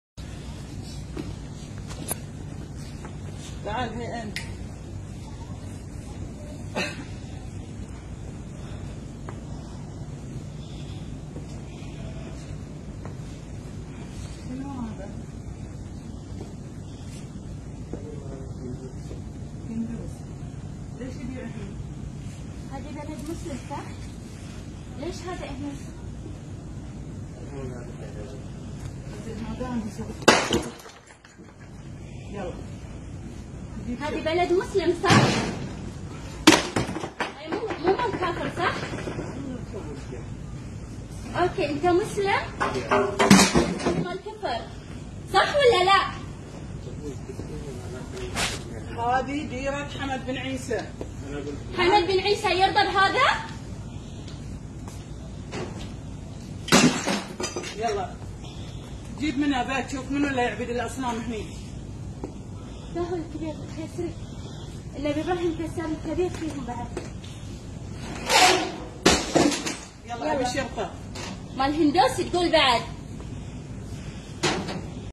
Bahrain Hindu statues been broken by Muslim girl | Masha Allah | zindabad Islam
Islam haq ka deen hai